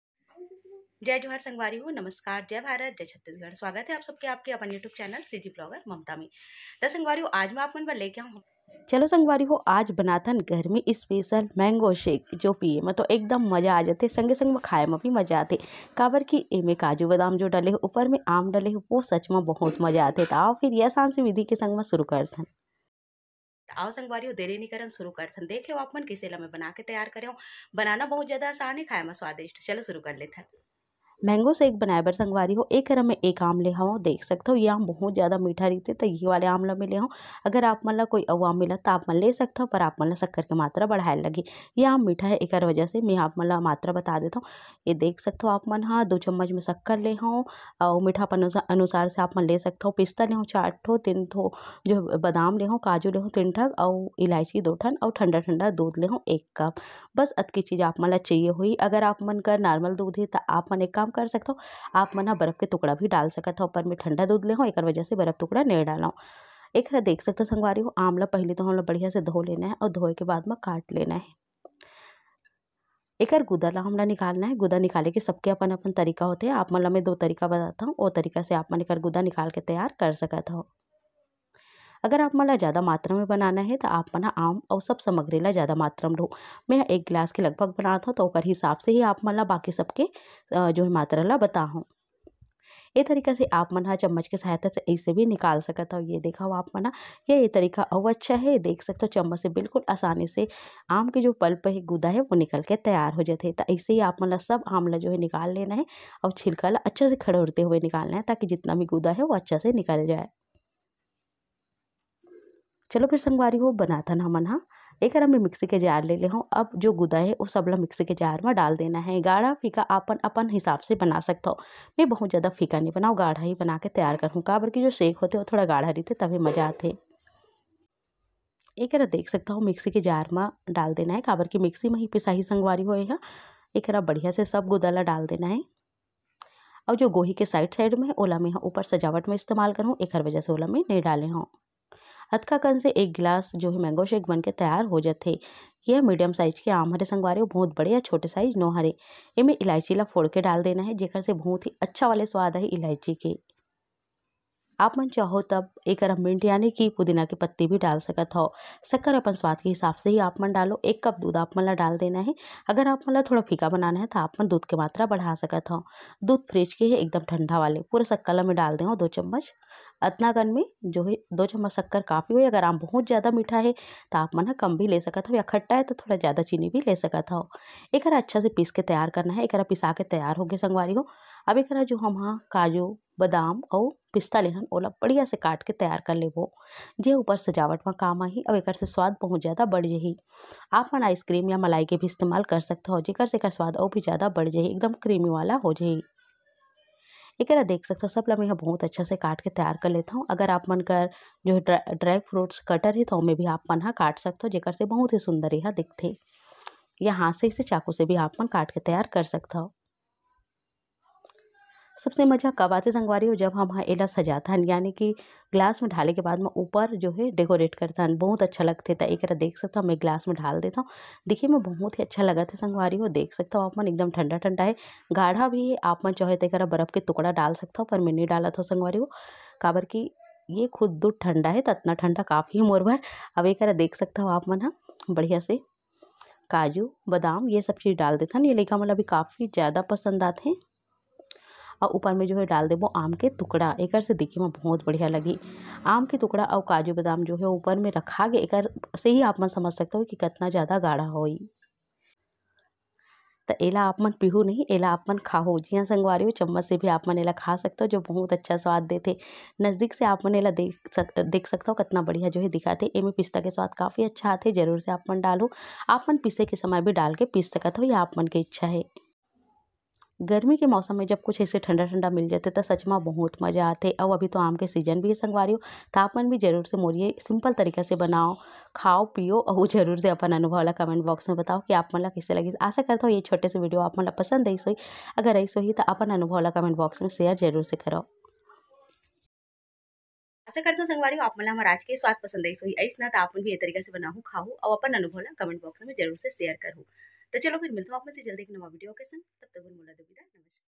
गर्मी में ठंडा ठंडा आम रस || how to make mango shake || cg vlogger Mamta
abaut this video :- is video me mai aap ke liye mango shake recipe le ke aai hu